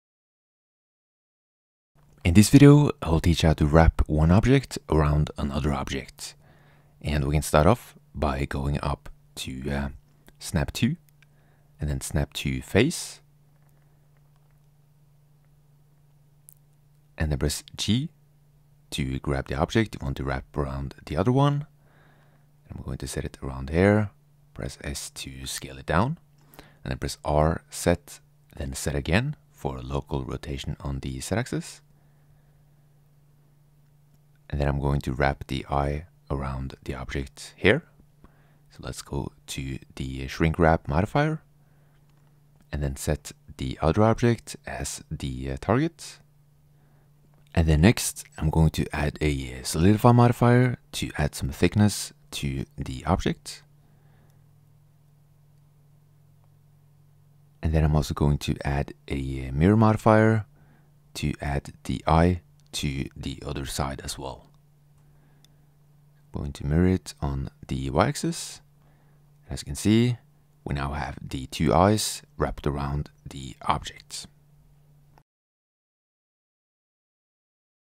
[2.93] Blender Tutorial: How to Wrap Objects Around Other Objects
Learn how to wrap one object around another object in Blender. Incredibly useful!